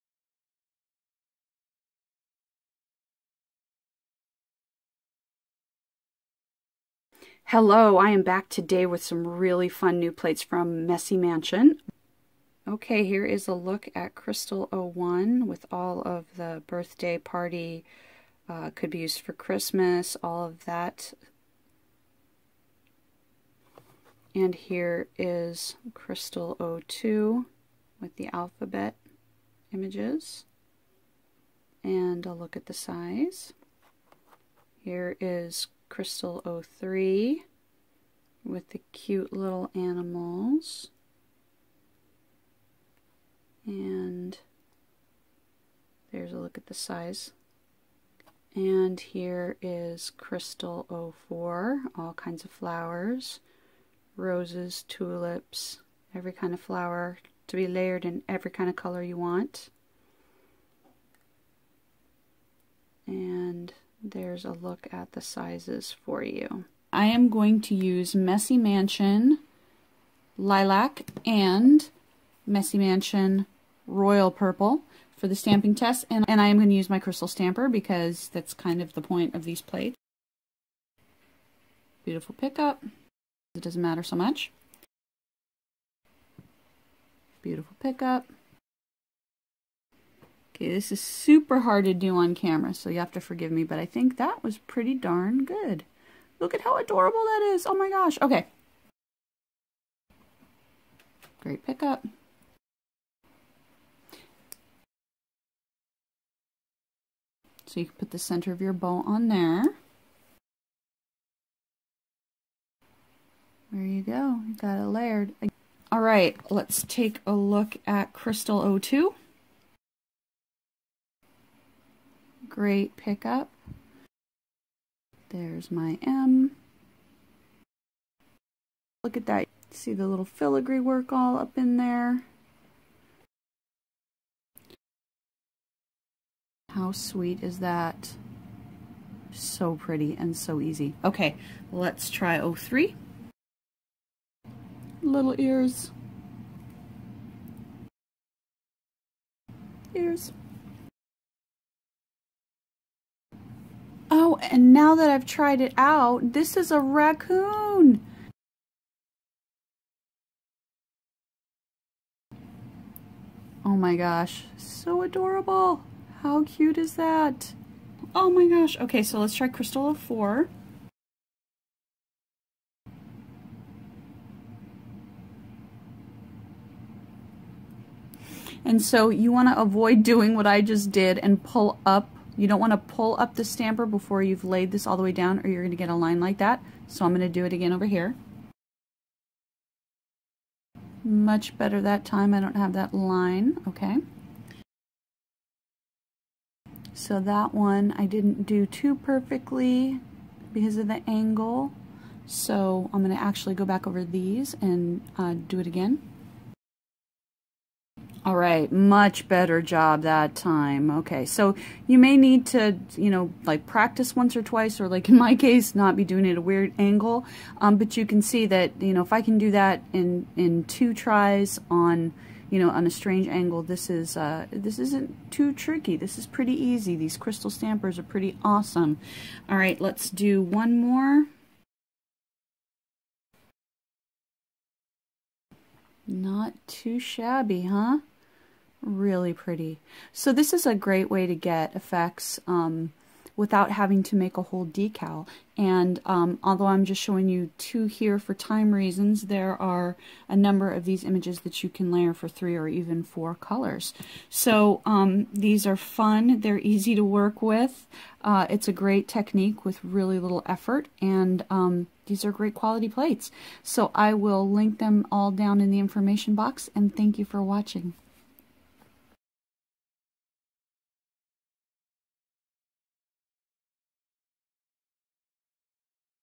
Quick review of Messy Mansion Crystal 01-04 Layering plates
Hello! This is my super-quick review of Messy Mansion's Crystal 01-04 layering plates, sent for my honest review. I show the plates and some swatches--for my discussion of the images, please see my full review. :)

You can find the plates, polish, and stamper in the Messy Mansion store